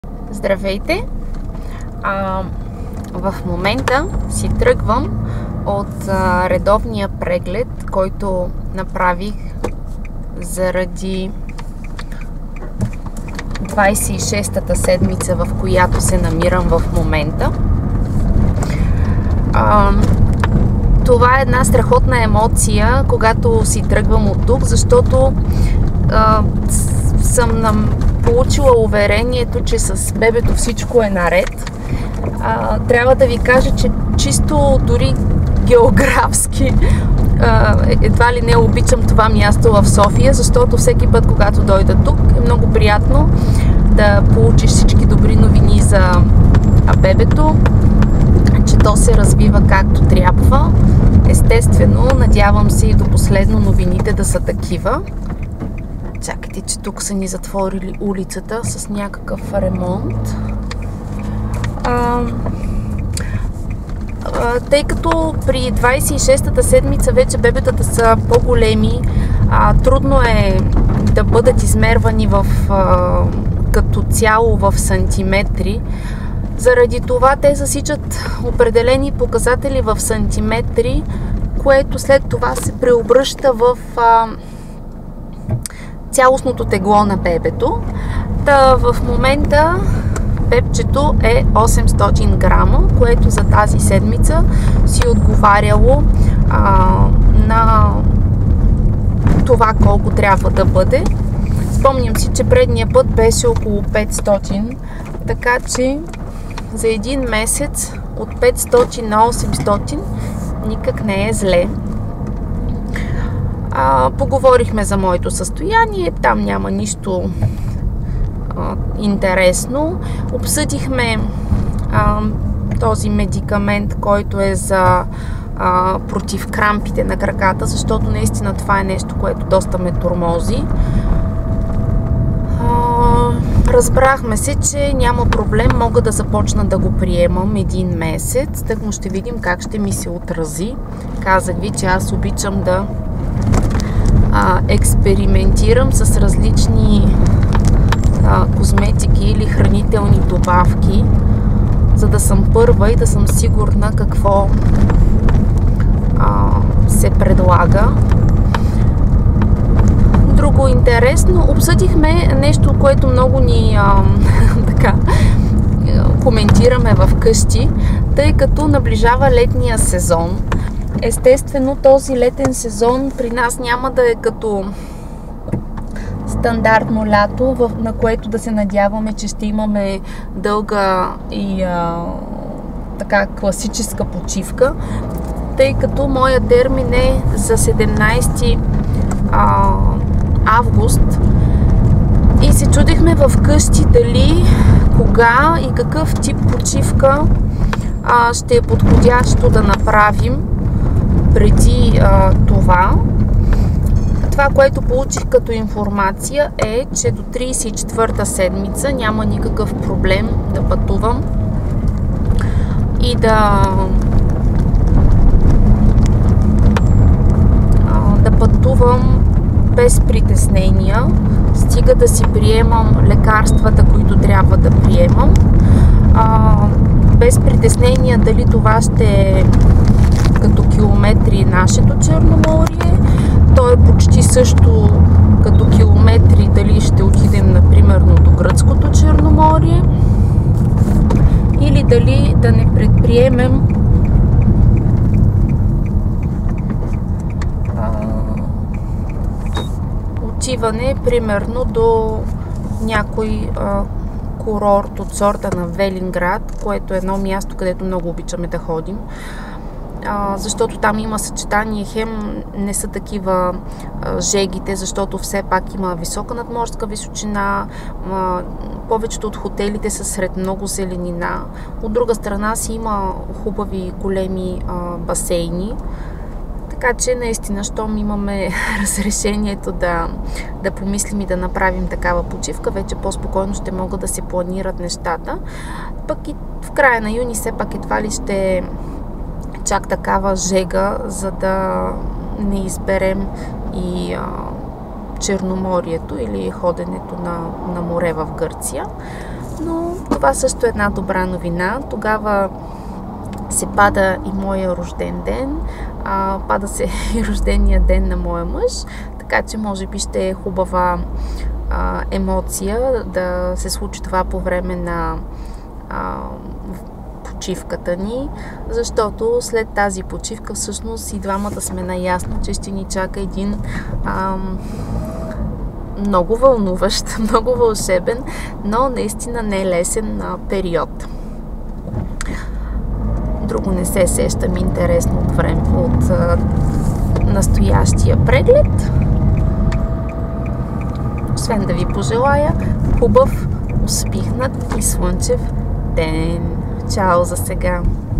След преглед в 26 гестационна седмица
Онлайн училище за родители - Dechko.bg, ви представя Боряна, която споделя с нас личните си усещания след прегледа в 26-тата гестационна седмица.